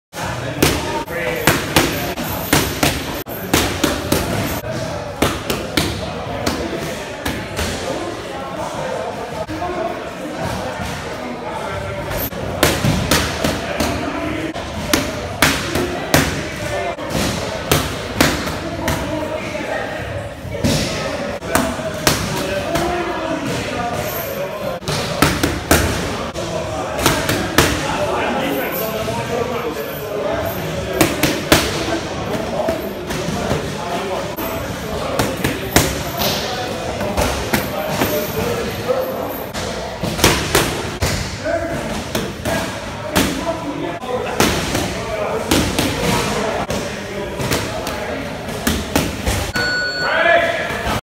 Amateur boxing drills
Great job as he prepares for his first amateur fight. A natural!